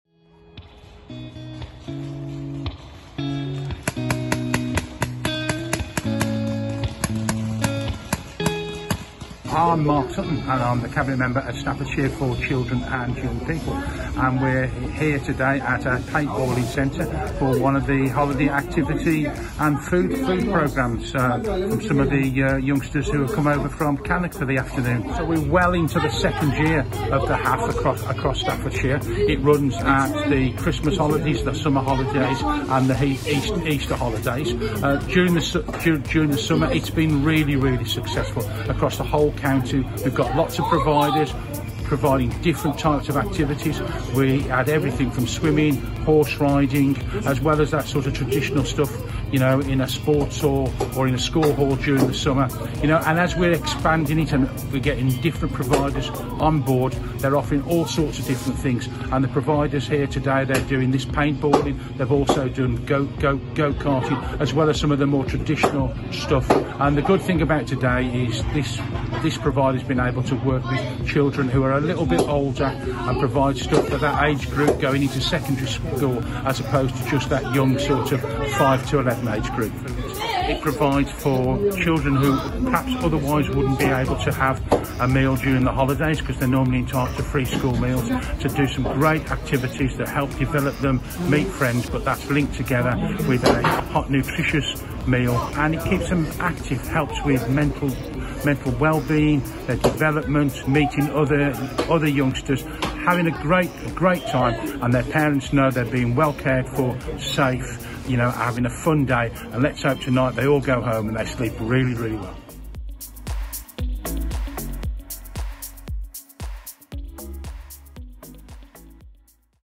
Holiday Activity and Food (HAF) | Paintball
Another successful Holiday Activity and Food (HAF) programme carried out this Christmas break 🎅

On Wednesday, children from Achieving Goalz and Dreams travelled from Cannock to @NPFBassettsPoleTV, to take part in a series of paintball games for HAF2022.

The children were all provided with a hot, nutritious and healthy meal, helping teams Orange and Blue battle it out throughout the day 💪